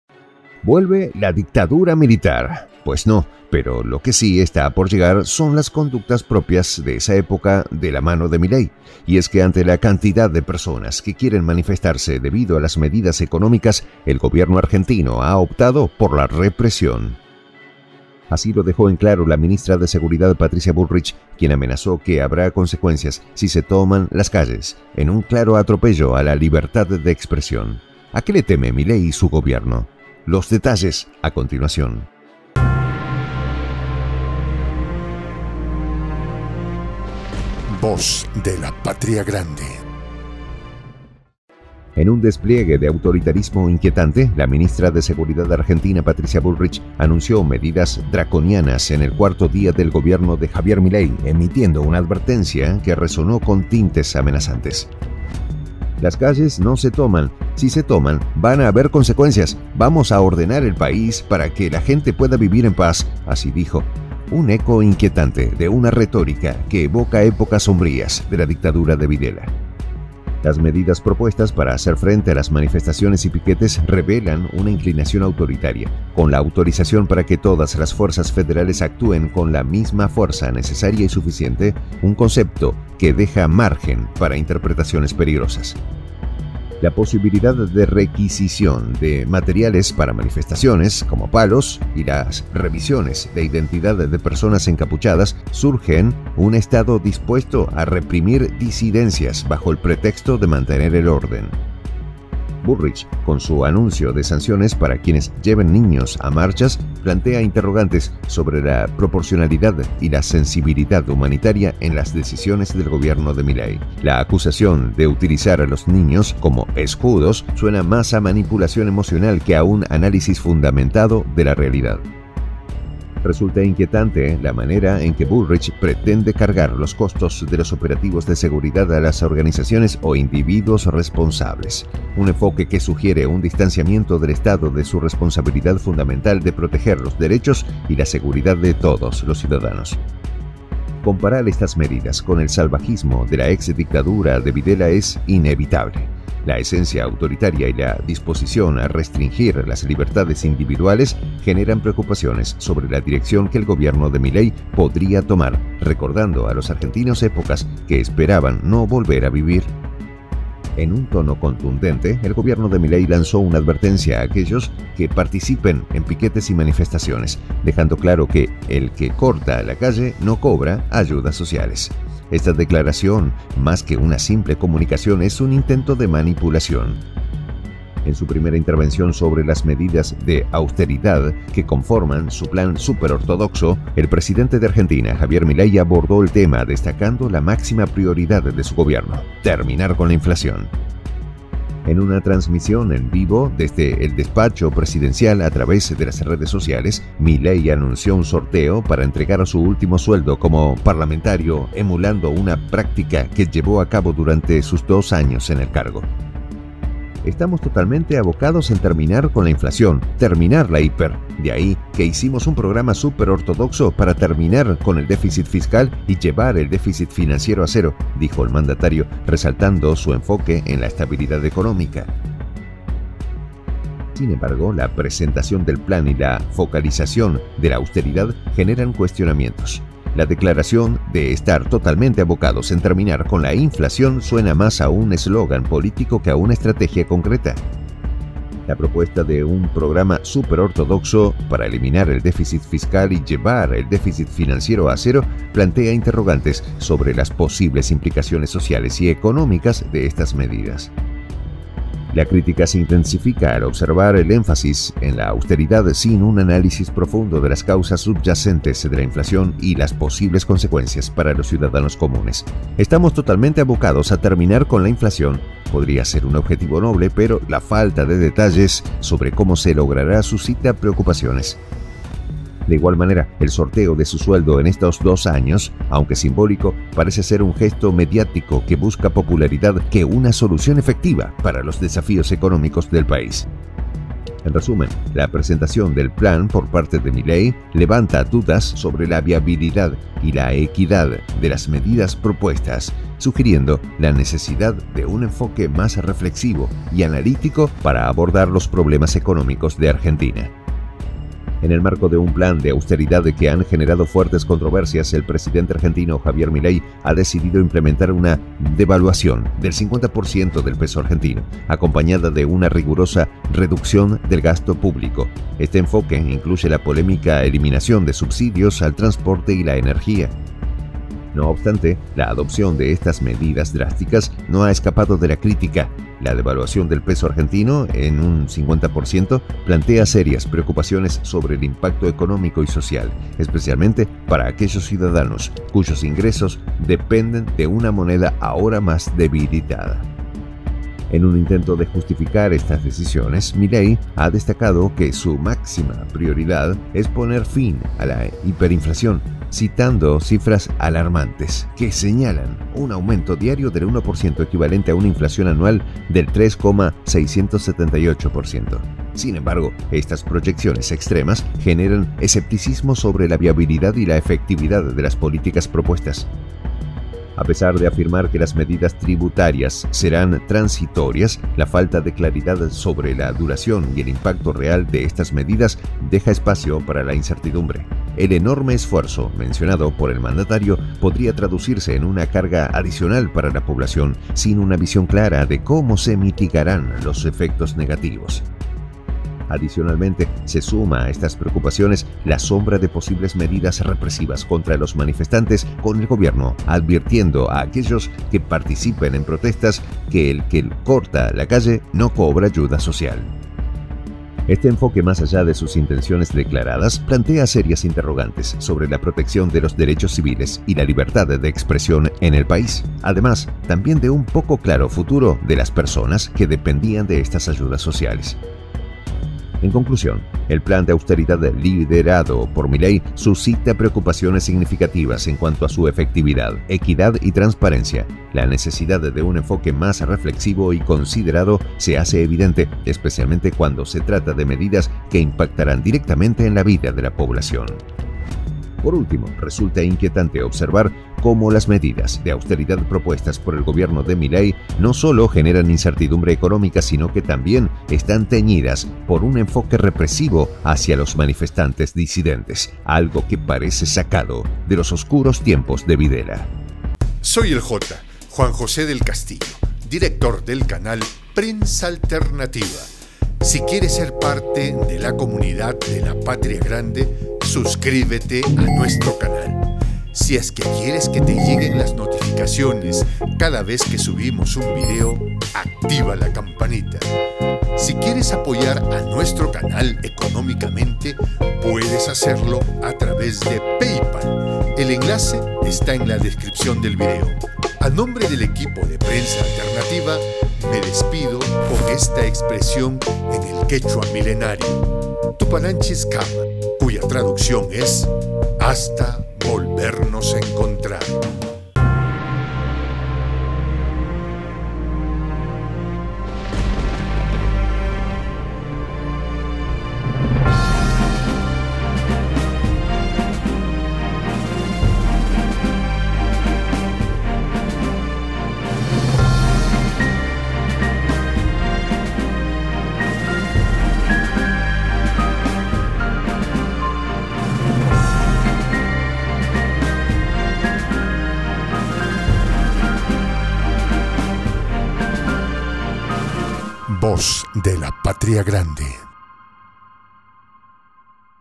Milei amenaza a su pueblo!
El mandato de Javier Milei ha empezado con el pie izquierdo. Y es que, tras presentar unas medidas económicas muy poco populares, el actual gobierno planea reprimir a los manifestantes. ¿Cómo se llegó a este punto?

Director General: Juan José del Castillo - El Jota
Jefe de Prensa: Enzo Anchante REV100001JP01
Redactor: Fabrizzio Torres

Org
JR1
L01
E03
dic2023PA